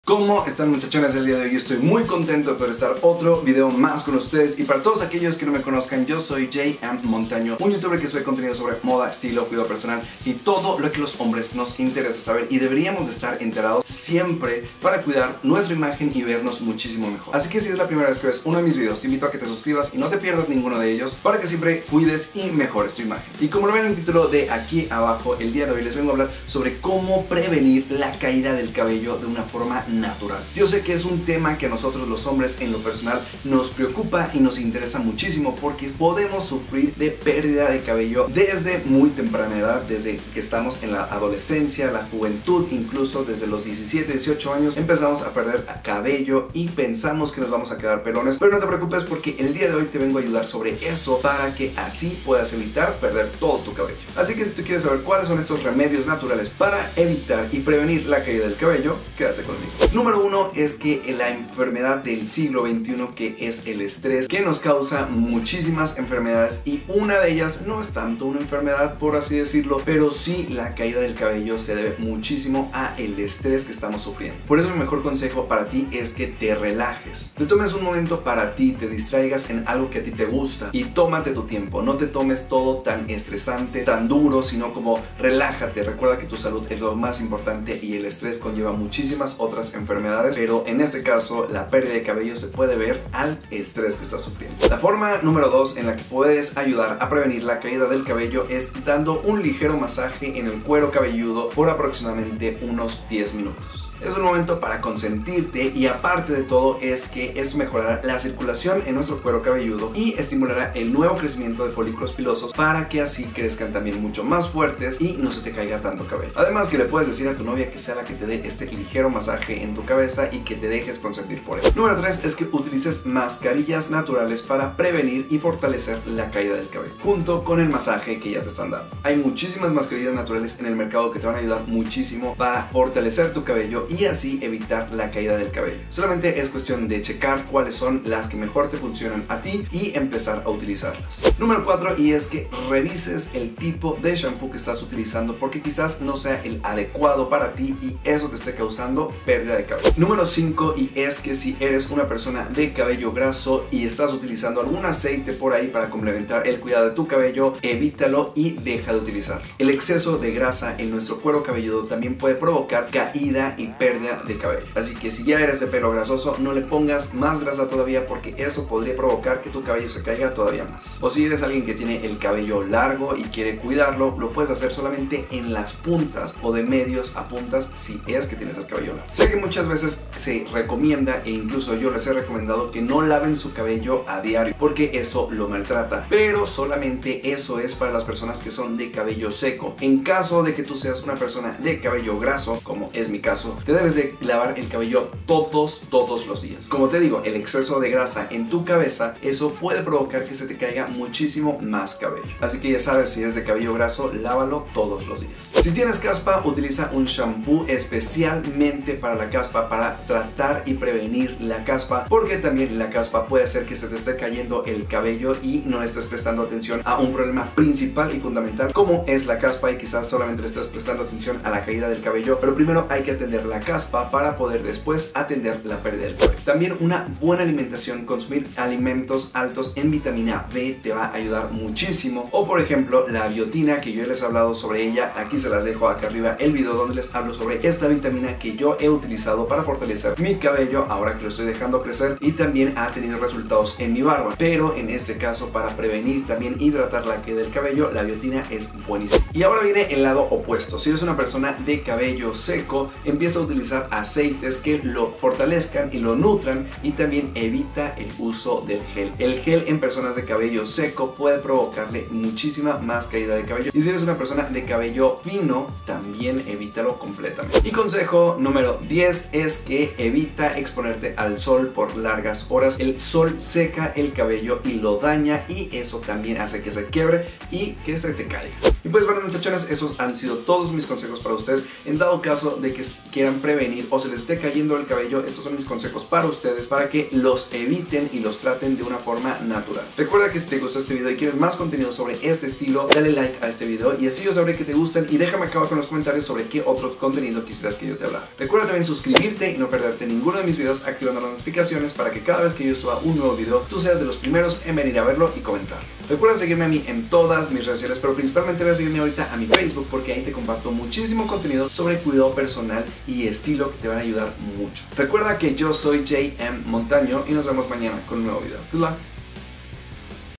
COMO DETENER LA CAIDA DEL CABELLO DE FORMA NATURAL - J.M. Montaño
Se que a muchos hombres nos preocupa la caída del cabello. Y en la actualidad existen muchos métodos naturales para evitar que esto suceda, e incluso poder detenerlo.
Por eso hoy te comparto formas sencillas de como puedes detener o evitar la caída del cabello, y no quedarte pelón.